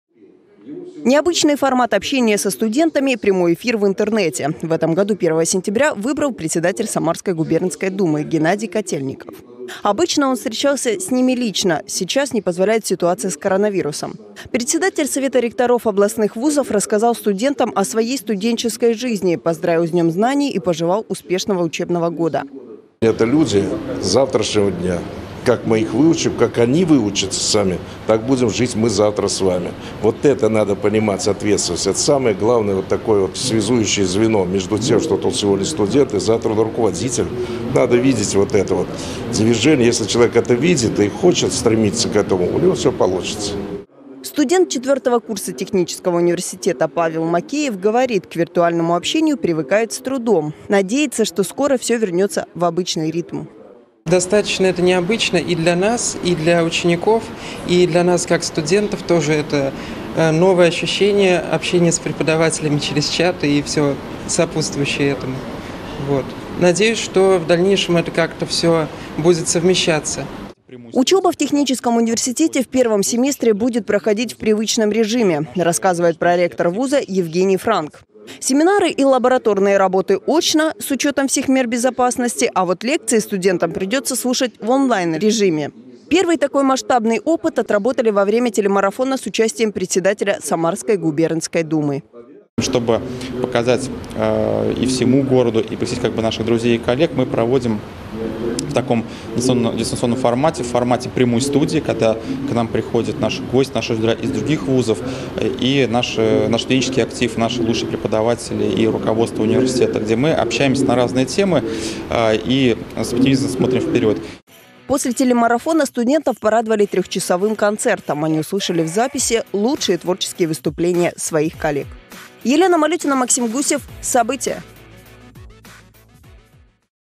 Председатель Самарской губернской думы поздравил студентов технического университета с днем знаний
Председатель Самарской губернской думы поздравил студентов технического университета с днем знаний. Геннадий Котельников в этом году встречался с ними в он-лайн режиме. Что академик российской академии наук пожелал сегодняшним студентам расскажем в нашем следующем сюжете.